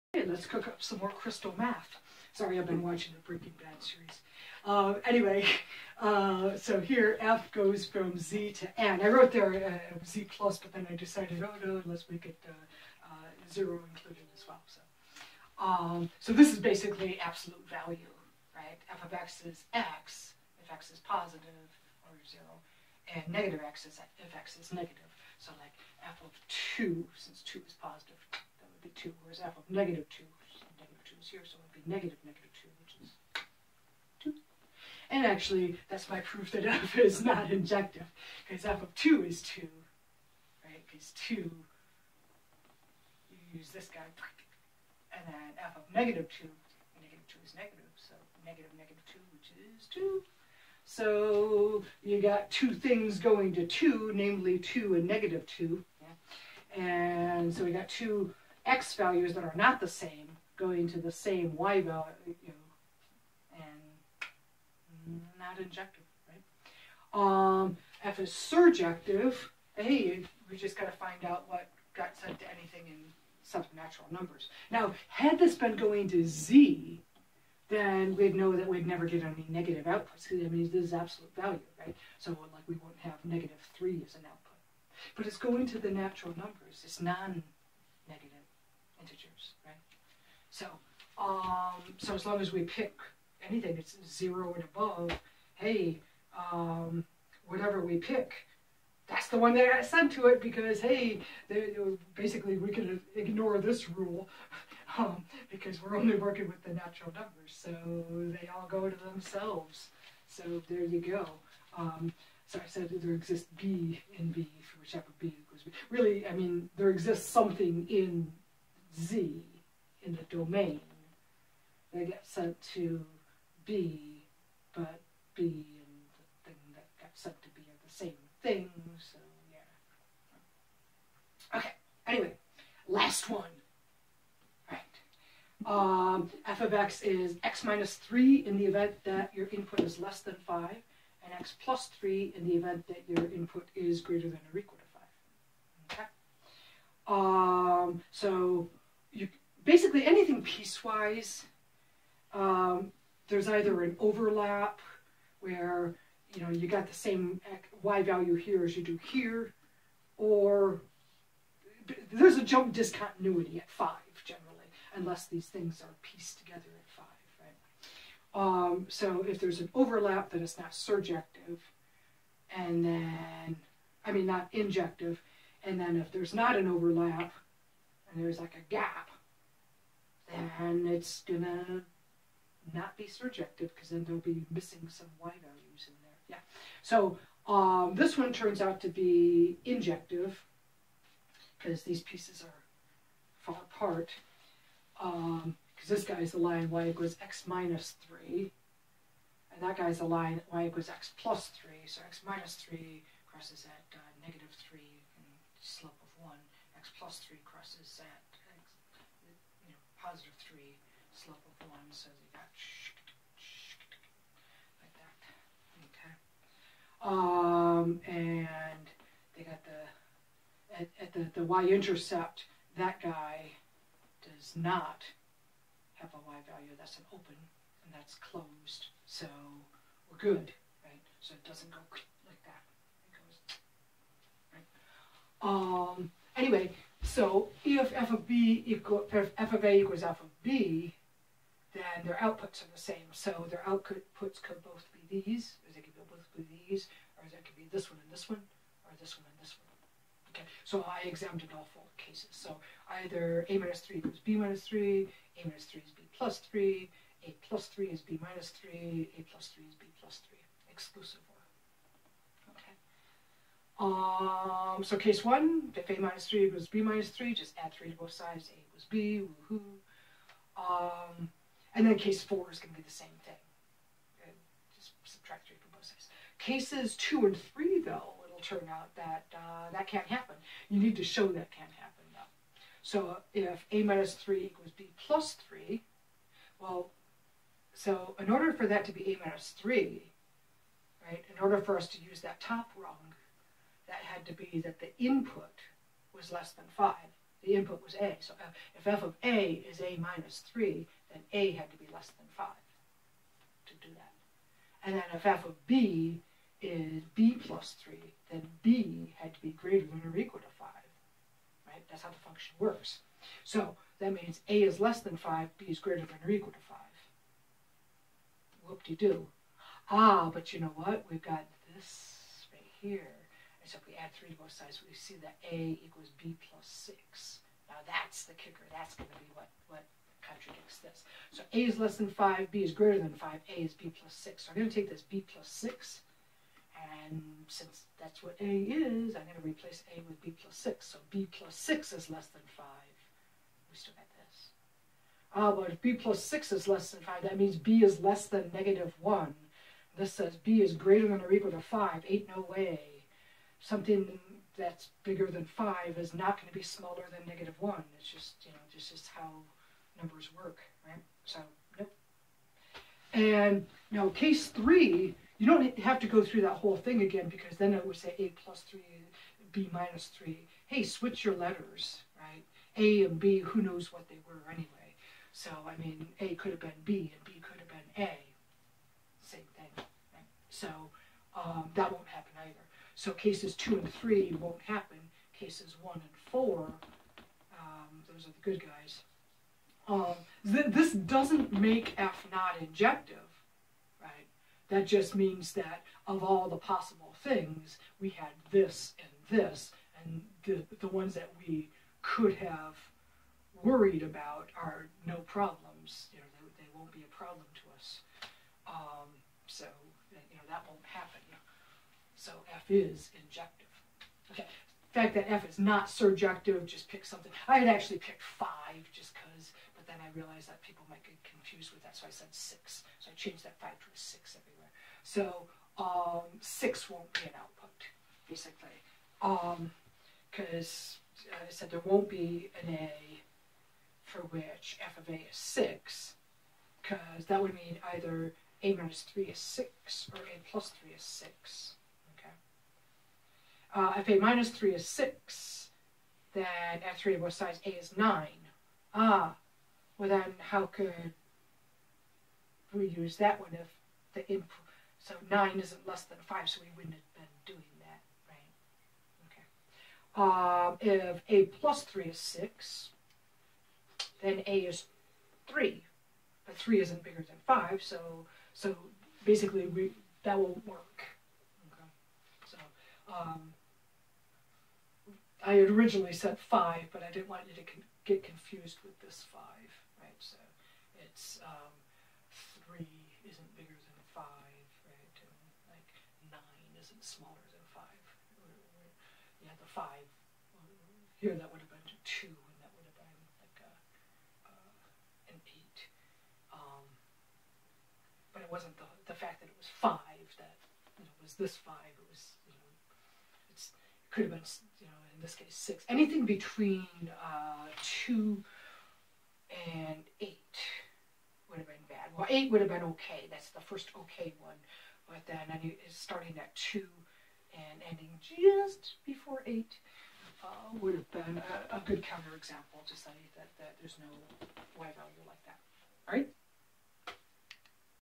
Proof that Z and N have the same cardinality
This is a proof that the set of integers has the same cardinality as the set of natural numbers